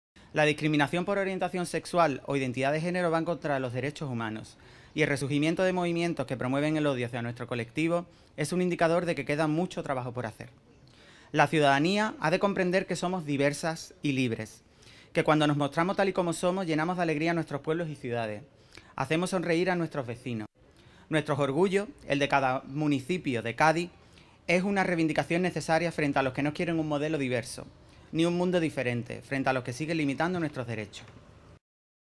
Las administraciones públicas de Cádiz izan la bandera arco iris
En el día del Orgullo LGTBI se suma esfuerzos con los colectivos para exhibir este emblema de la tolerancia e igualdad